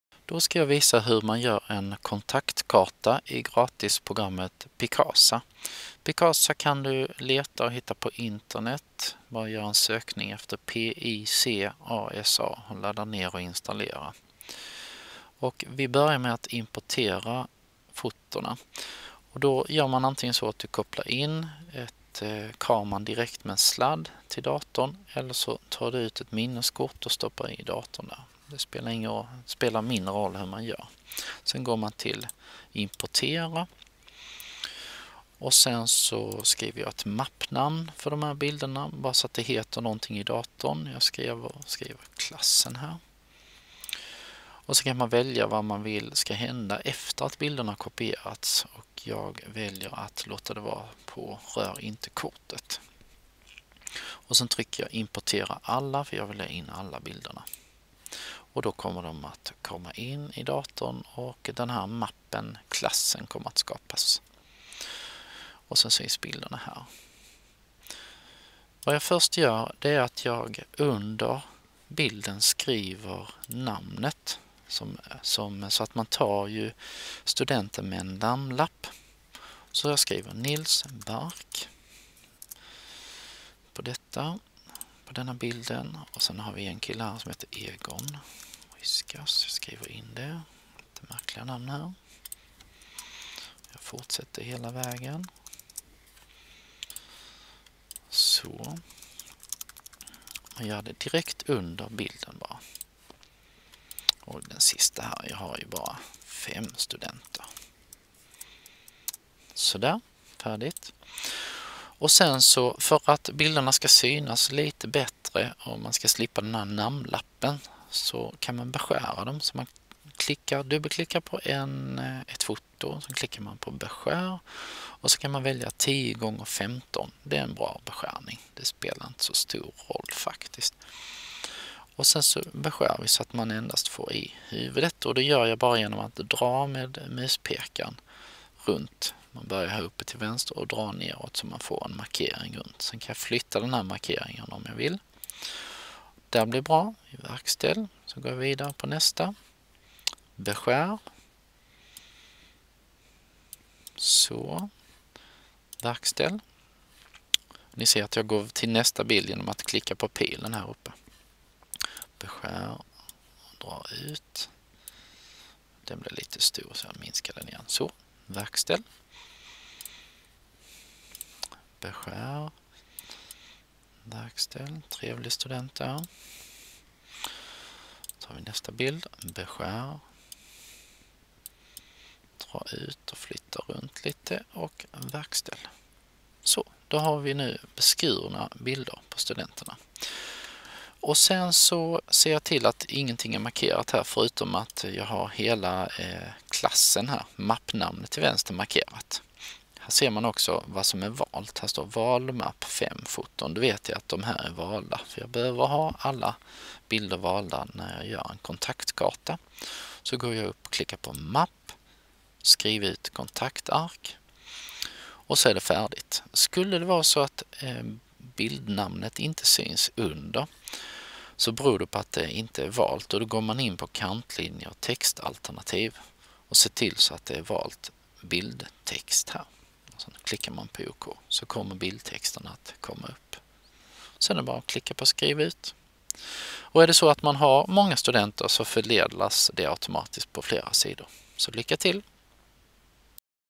Hur du skapar kontaktark i Picasa
Jag beskriver hur du skapar kontaktark i fotoprogrammet Picasa; ett gratis program som du själv kan ladda ner och installera.